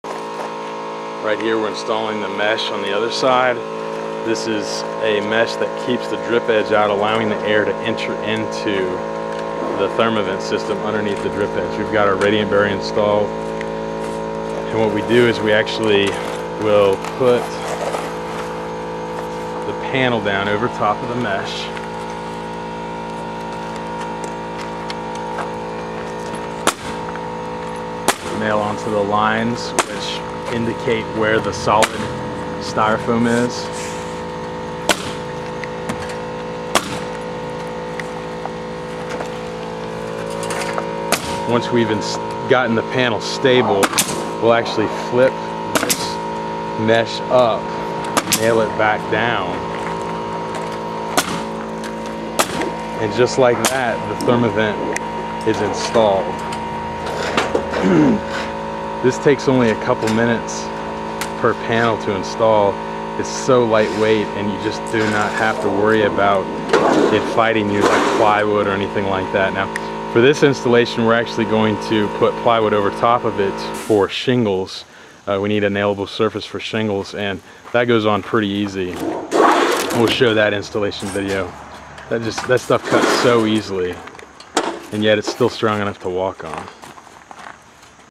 Therma Vent Installed with Mesh 3
This is a close up of the install of the Therma Vent over the Mesh, this allows for an air gap and air to move over the bottom and the top with both the air channels and the small lines on top.
ThermaVentilation.com